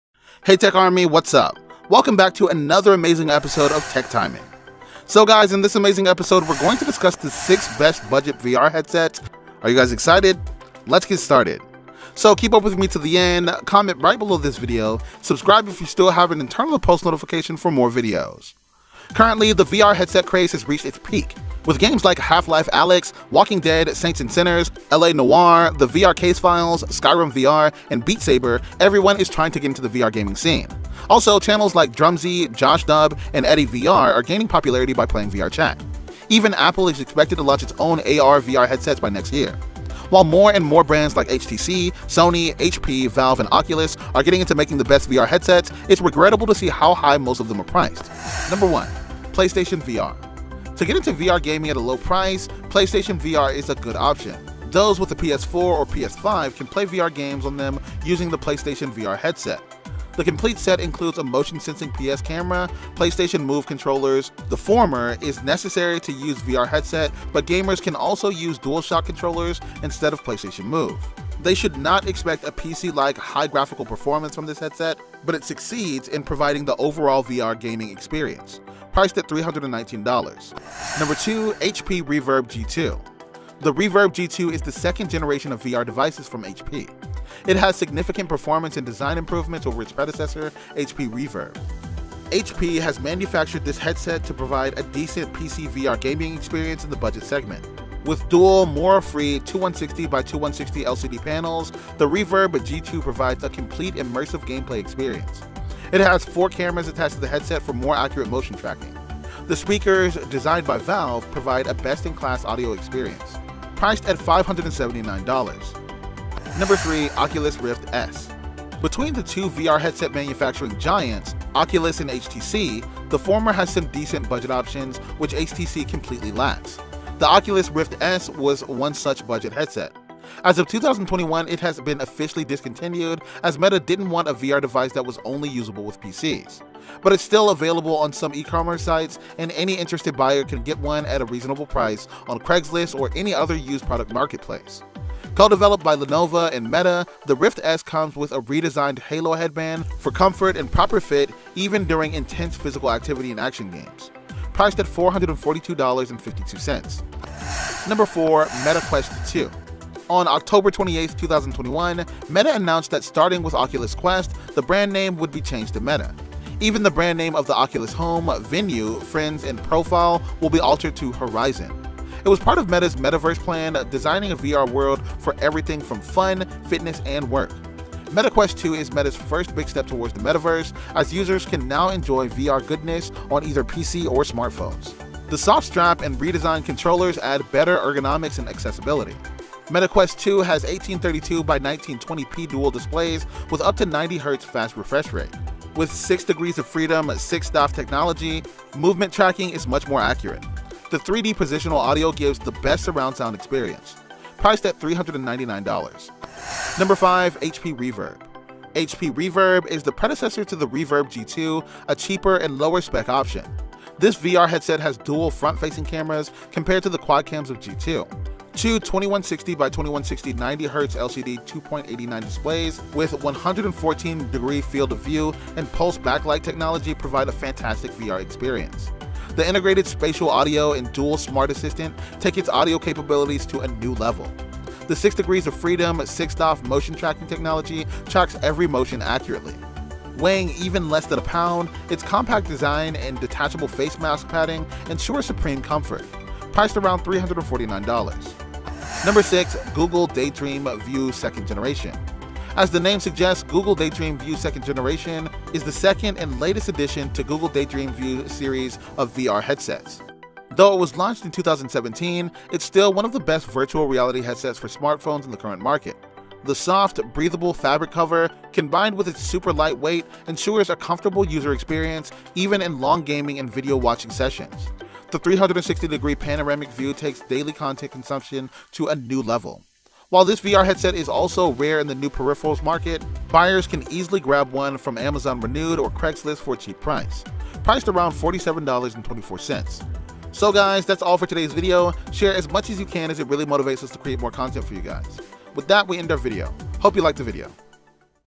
6 BEST BUDGET VR HEADSETS OF 2022!
6 BEST BUDGET VR HEADSETS OF 2022!

In this video, we break down the best budget VR headsets of the year!